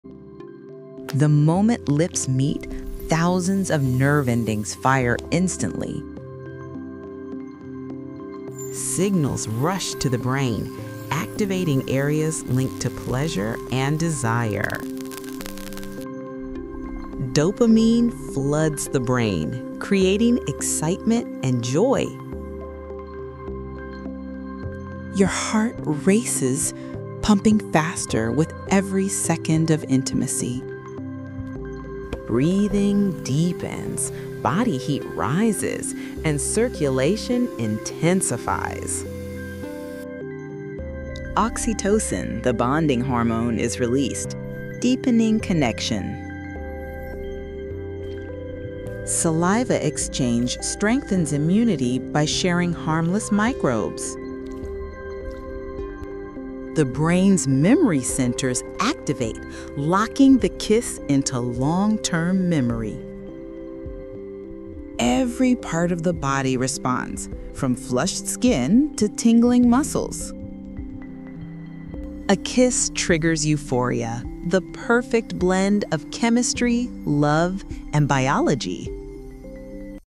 What Really Happens During a Kiss | Science of Love in 3D Explained
What actually happens inside your body during a kiss? 💋
This cinematic 3D animation reveals the real science behind kissing — from nerve activation and brain chemistry to heart rate, hormones, and emotional bonding.
Learn how dopamine, oxytocin, and endorphins create feelings of happiness and connection, why your heart races, and how kissing can even help reduce stress.
This video is for educational and scientific purposes.